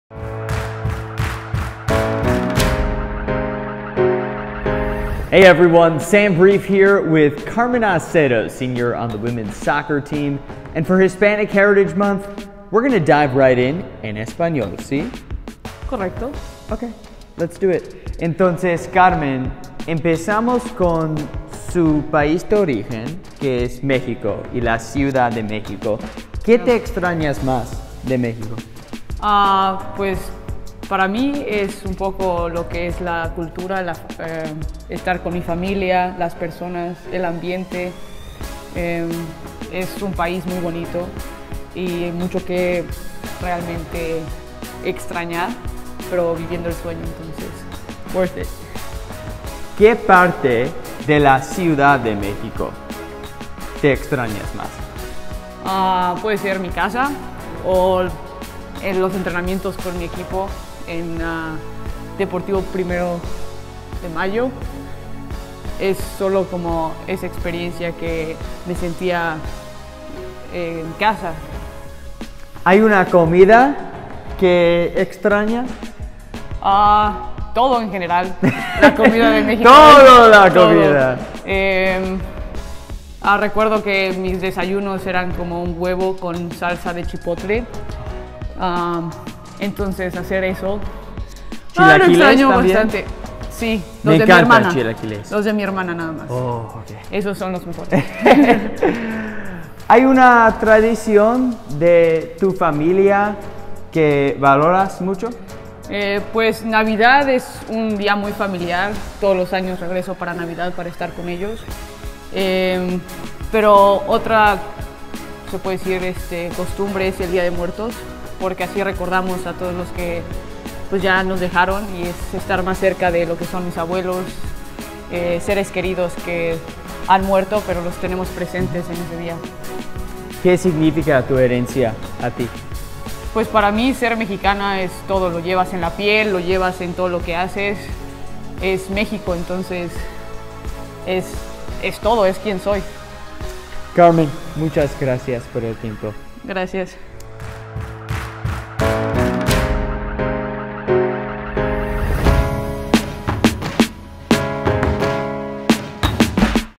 Celebrating Hispanic Heritage Month with Carmen Acedo
Chicago State women's soccer defender, a Mexico City native, chats with Sam Brief about her heritage.